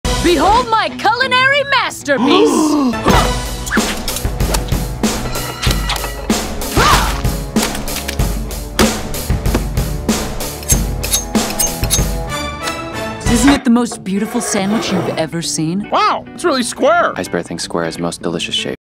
Chow Down With The Bears On Tabe's Spectacular Sando!😋
This video contains a recipe for Ranger Tabe's Sandwich From We Bare Bears! Watch this video Ranger Tabe's Sandwich From We Bare Bears and see how it is done.

➡️Beacons.ai
🔗beacons.ai/cookingwithfred